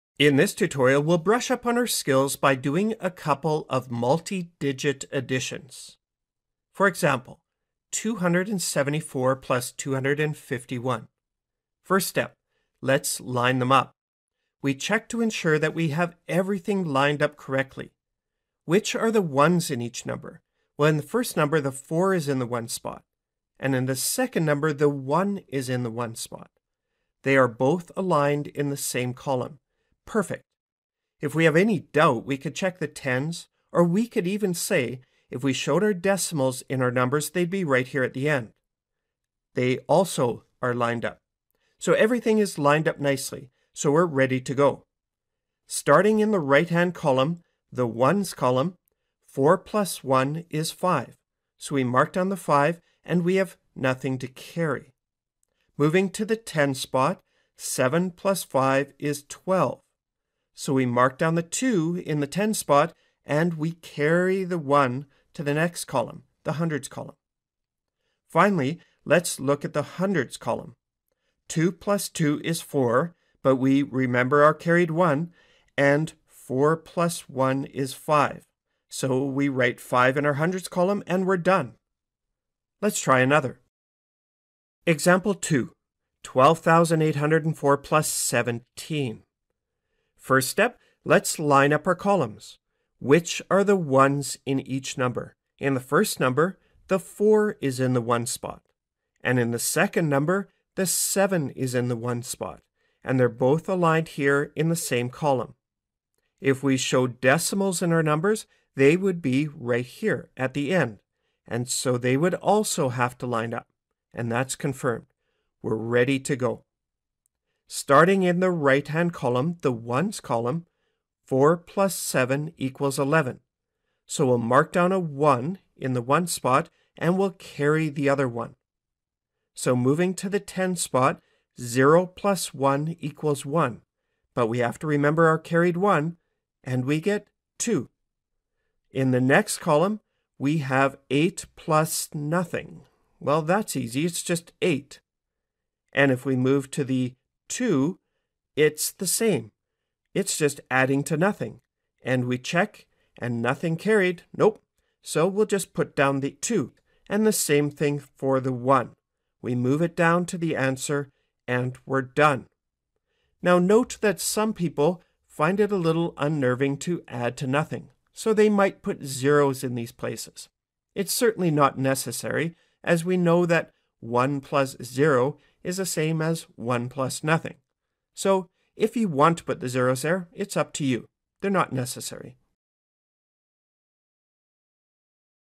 WCLN - Math - Adding Multi-Digit Numbers & Carrying
In this tutorial we demonstrate how to add multi-digit numbers.